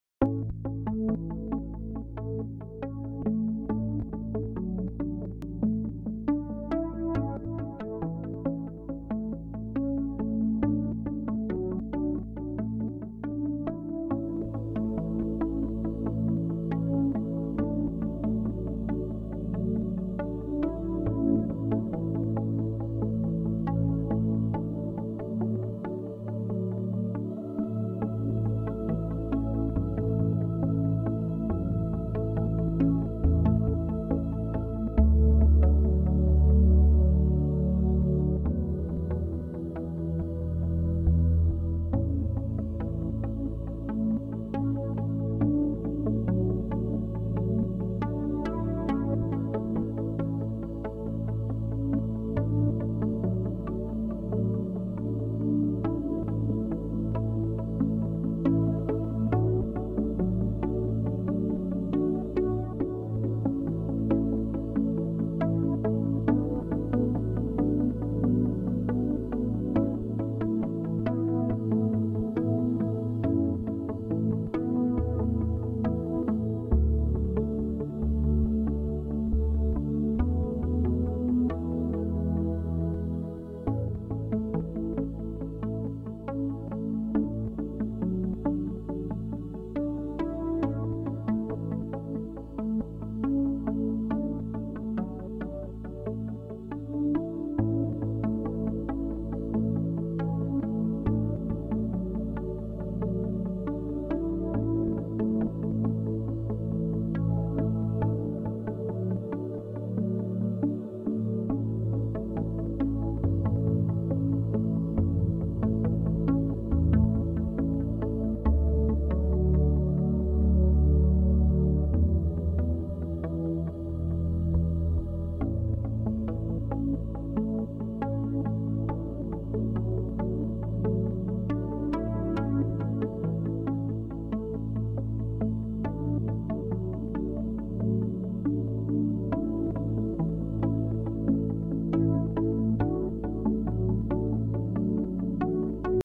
Object Shorts I - Episode 3b: "Hurt the Fan"
Flower kills Fan and Test Tube out of anger.